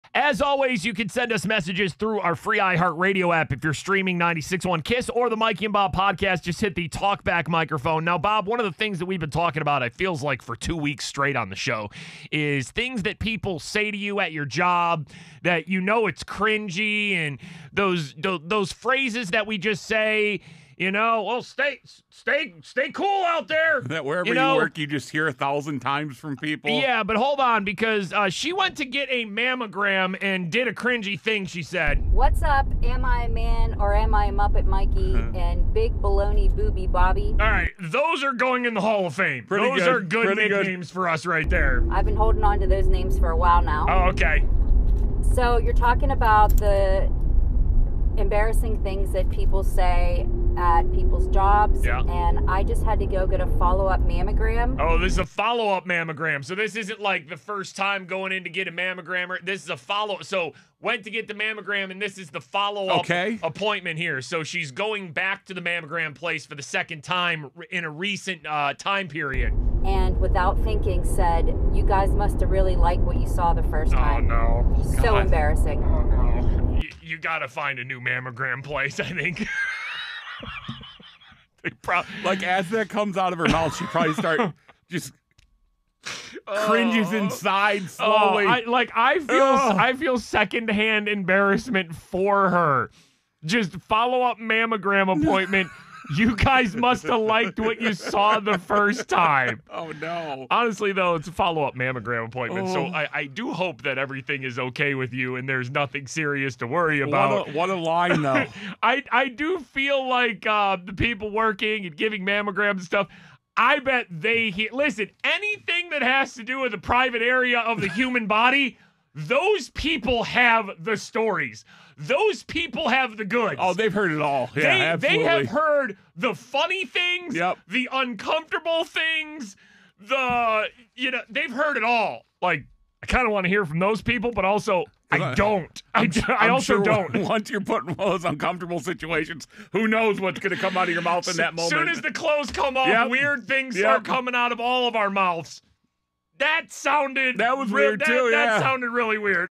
Cringy line said at the mammogram appointment
A listener shares her cringy moment when getting a follow up exam